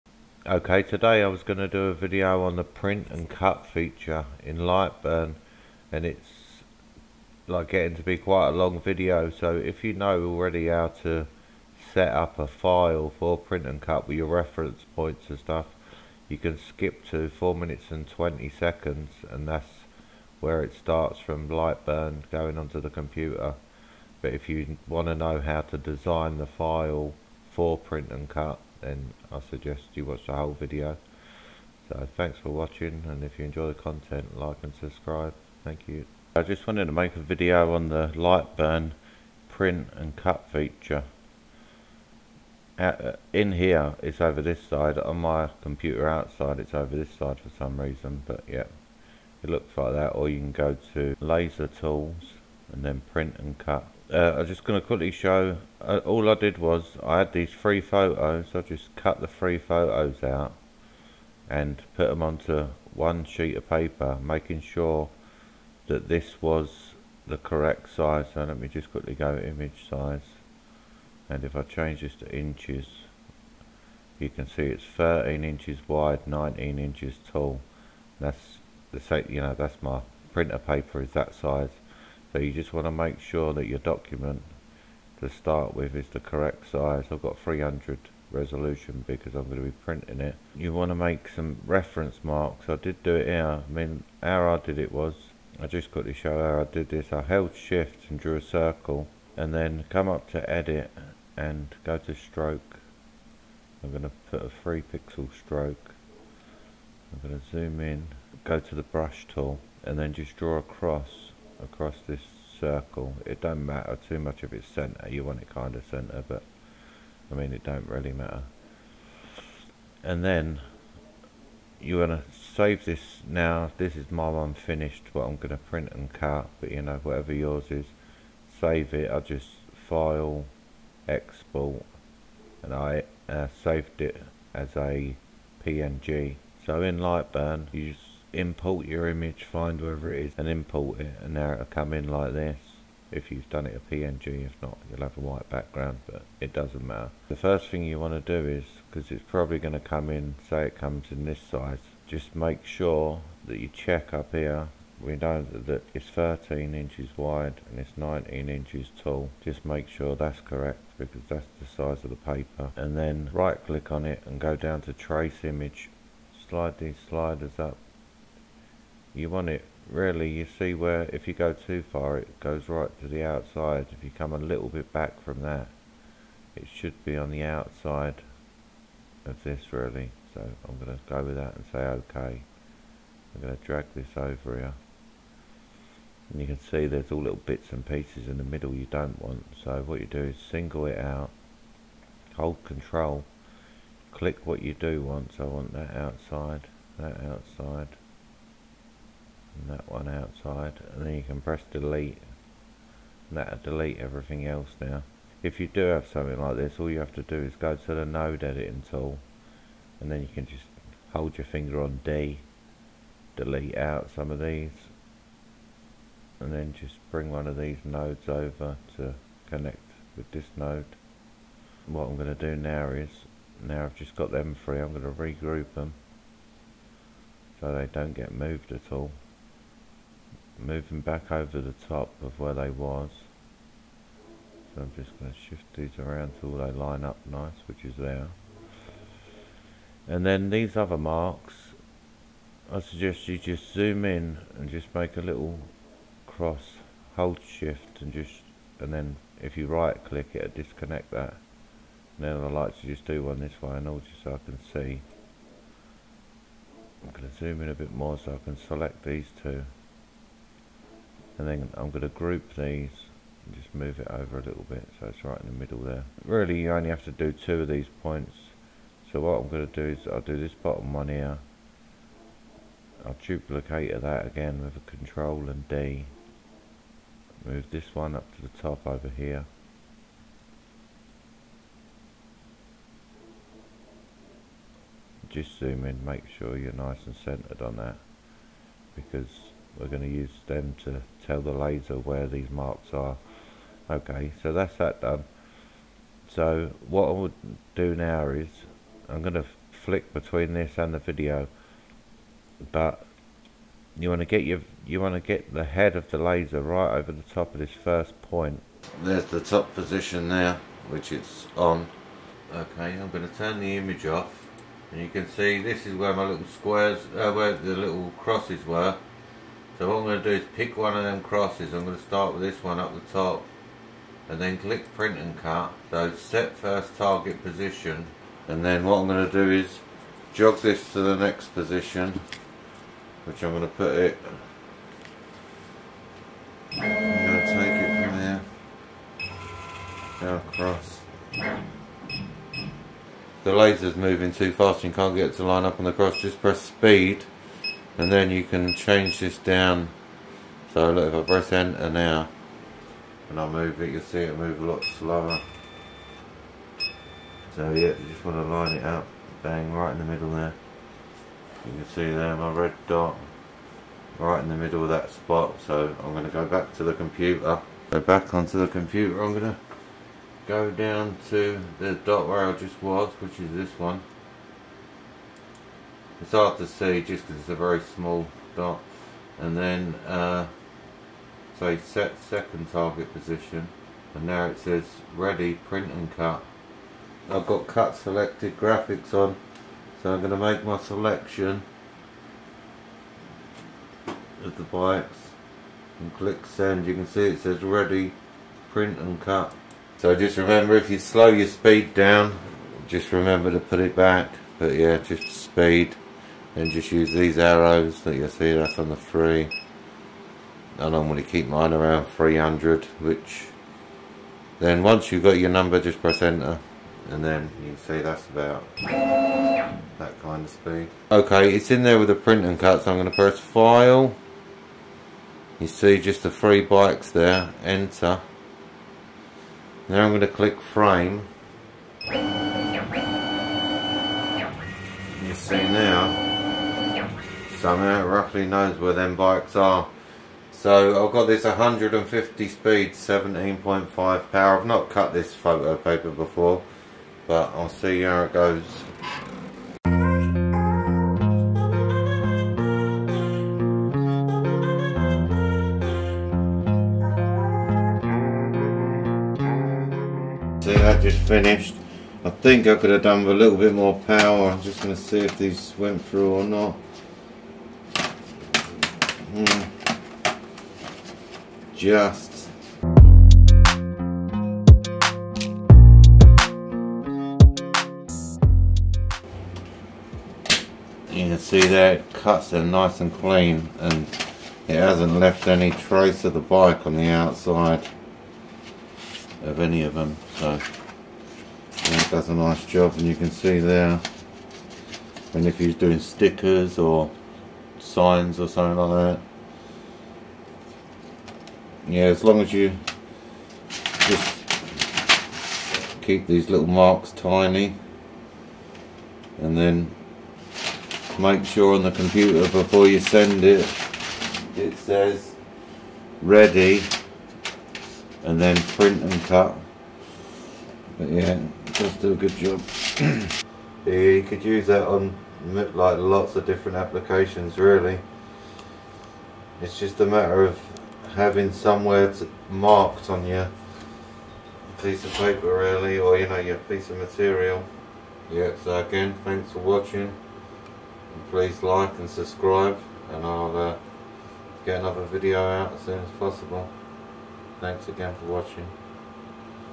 how to use Lightburn Print and Cut Feature to Laser cut anything Perfect Co2 Laser Cutouts
0:00 How to design
4:20 Print and cut

What is the print then cut feature?
Image result for print and cut feature
Print then Cut is an option that allows you to print your designs and then cut around them. When you have an element or design set to “Print then cut,” Design Space will send it to your home printer first, and then it will cut it